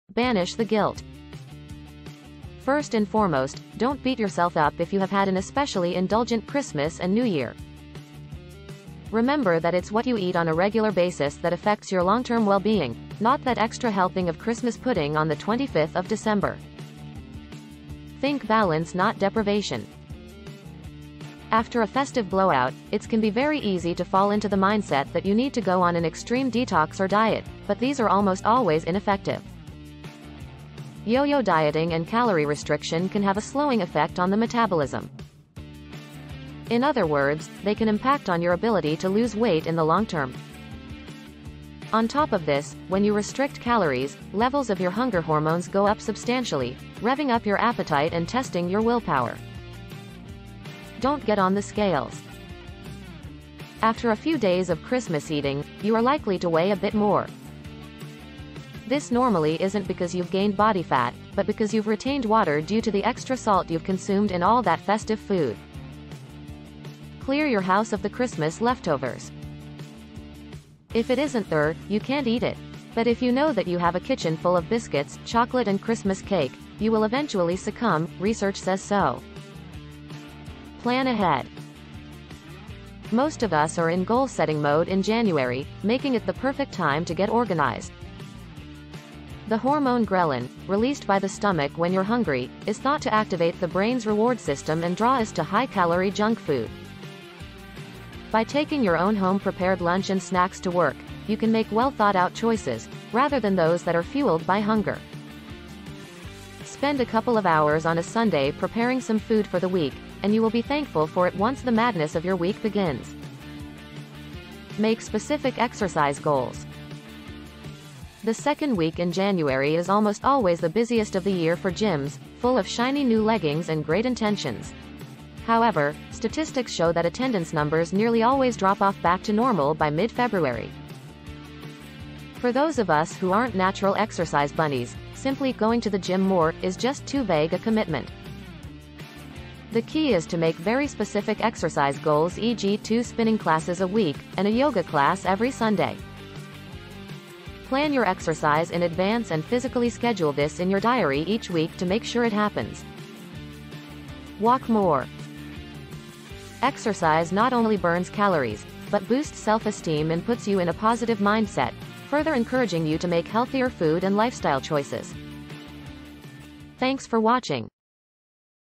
how to lose weight after christmas
After all the excesses of the festive season, it is unsurprising that one of the most commonly vowed New Year resolutions every year is to lose weight. If dropping a few pounds is high on your list, or you simply want to get a bit healthier, here are some top tips to support you this January…